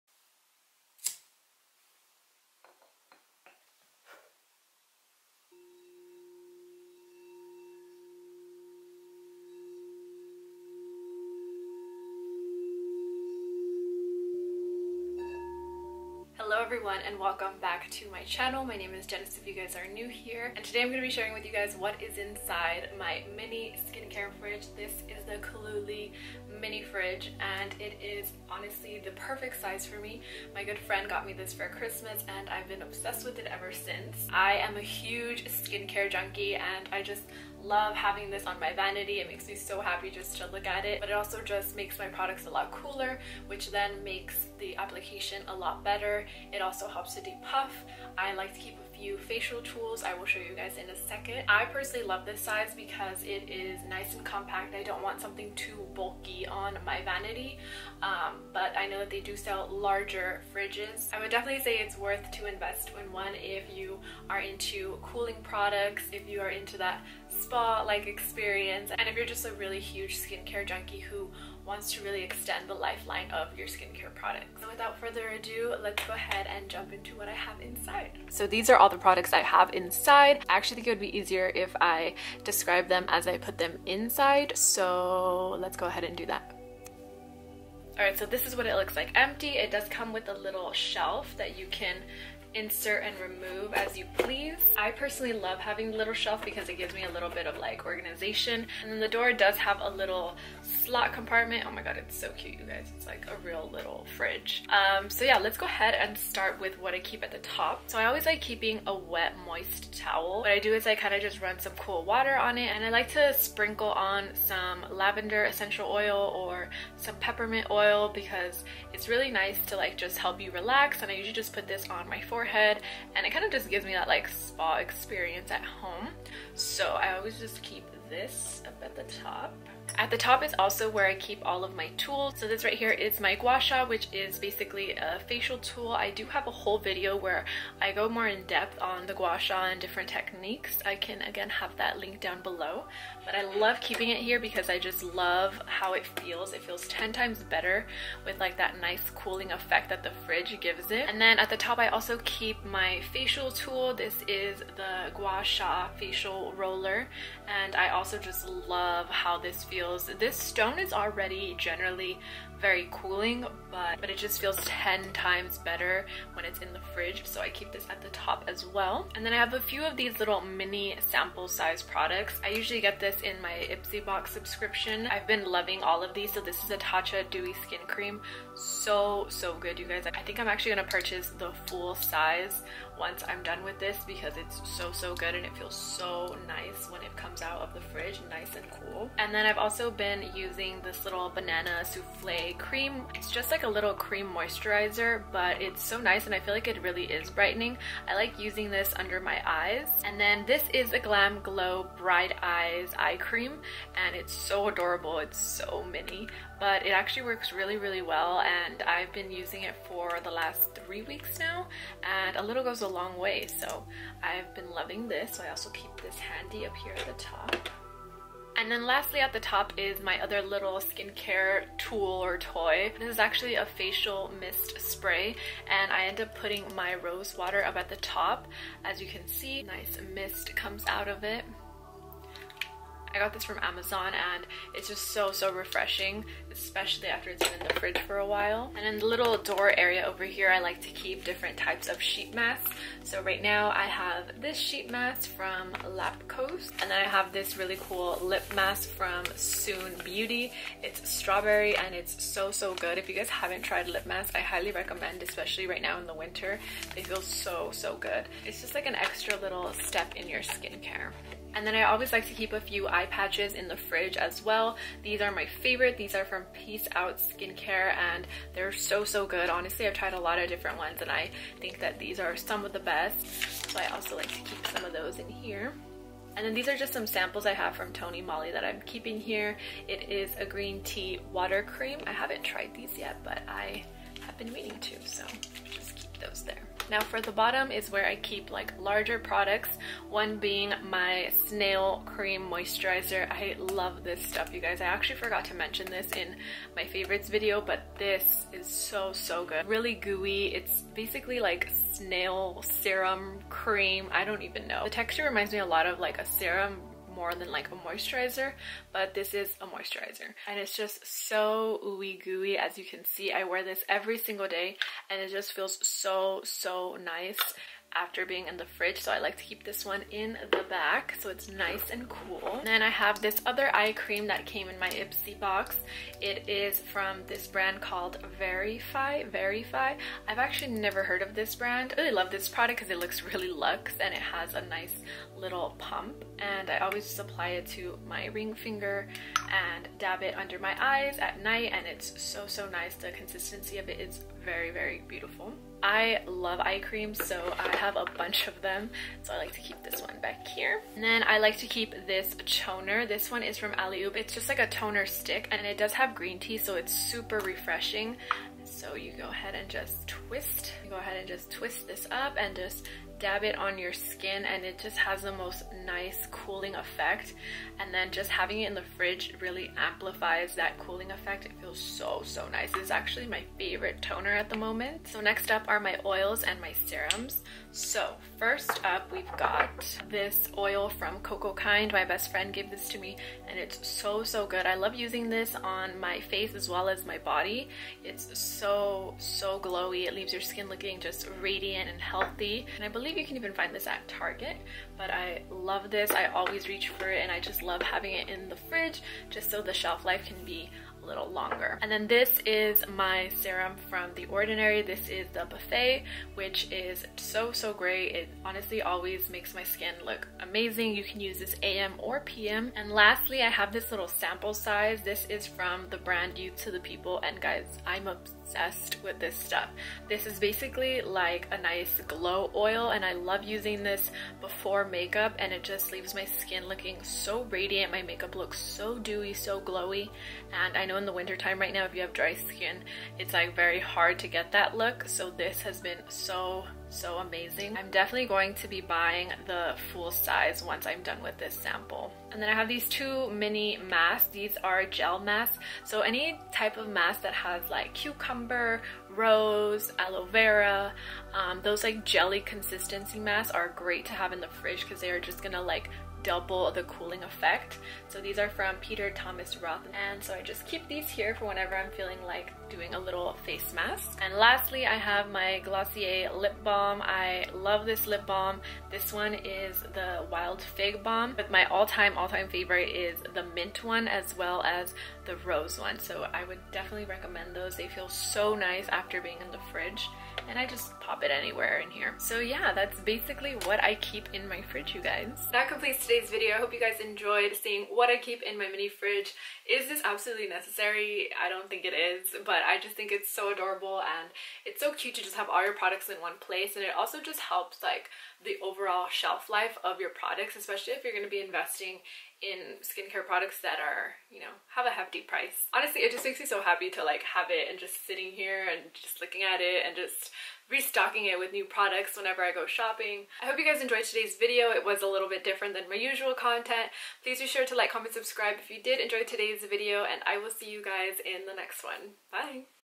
WHAT I KEEP IN MY SKINCARE FRIDGE
Sharing what I currently keep in my mini skincare fridge ~ skincare is such a relaxing experience for me so wanted to bring you into my little ritual.

Products Mentioned:
Gua Sha
Jade Roller
Tatcha Dewy SkinCream
Banana Moisturizer from Glow Recipe
GlamGlow Eye Cream
COSRX All in One Snail Cream Moisturizer
The Ordinary "Buffet" serum
Youth to the People Berry Glow Oil
Alleyoop Toner
Peace Out Skincare Eye Patches
SoonSkincare Lip Mask